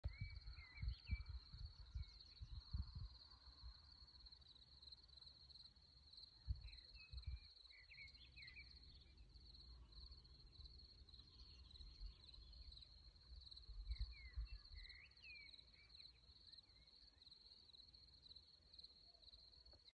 RELAX NATURE GERMANY *TAKE A DEEP SHORT BREATH*
Short Relax Video with beautiful nature bird sounds. Enjoy and relax your mind!